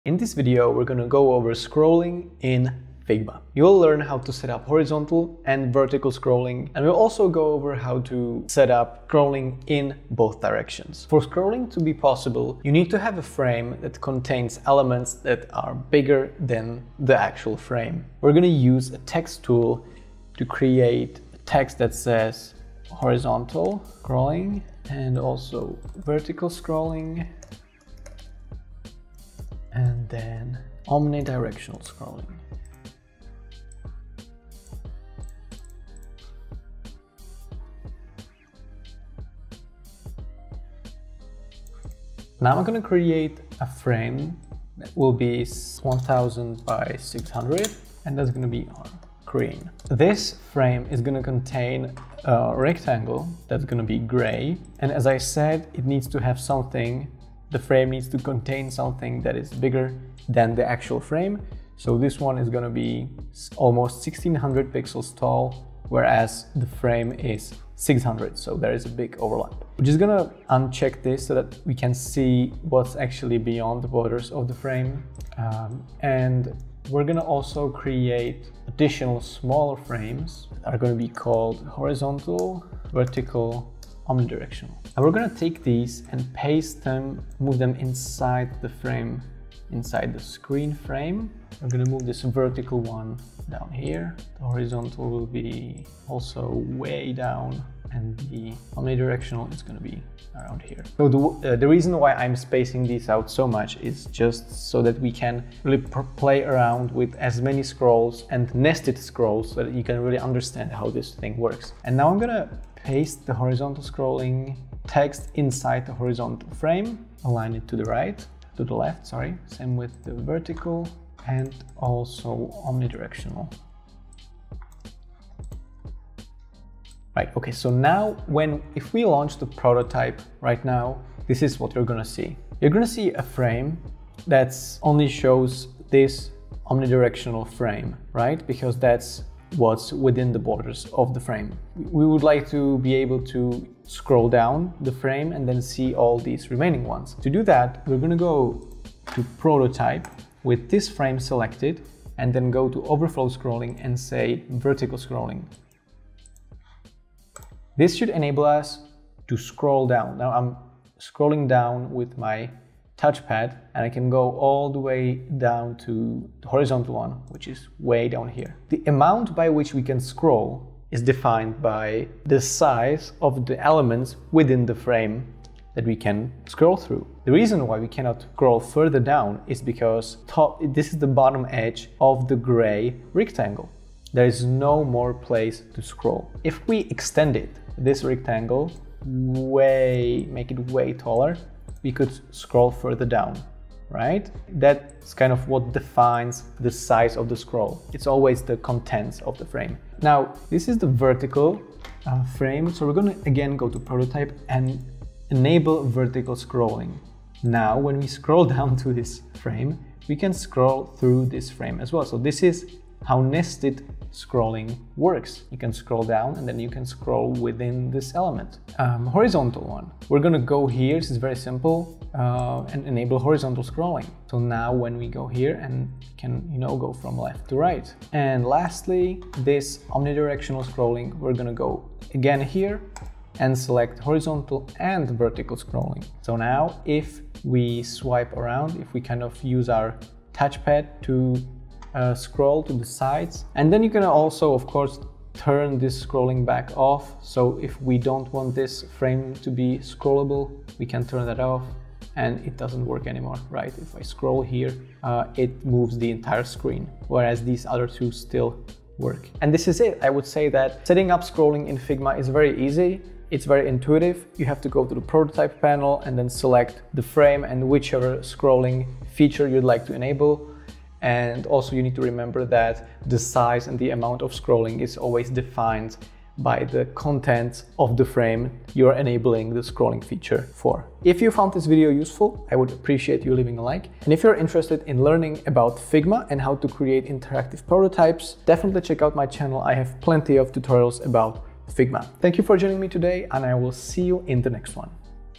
Figma SCROLLING (Horizontal, Vertical, Both Directions) Explained – With Examples
Let's learn about scrolling in Figma. In this video we'll go over everything related to this feature, how to set up vertical scrolling, horizontal scrolling, omnidirectional scrolling and also NESTED scrolling (frame scroll within frame scroll). The feature is called Overflow Scrolling and can be found in the Prototype panel.

0:00 — Intro & Structure
0:10 — What is necessary for scrolling to work?
0:18 — Setting up the page
2:09 — Result with scrolling disabled
2:28 — Setting up VERTICAL SCROLLING for the whole page
2:48 — What affects the amount of scrolling possible?
3:21 — Setting up NESTED VERTICAL SCROLLING
3:39 — Setting up NESTED HORIZONTAL SCROLLING
3:49 — OMNIDIRECTIONAL scrolling (horizontal & vertical scrolling)
4:04 — Disable scrolling
4:19 — Summary

In this video: Learn how to set up / use / create scrollable prototypes in Figma – Learn how scrolling in Figma works (+ nested scrolling) – With examples and explanation – easy to understand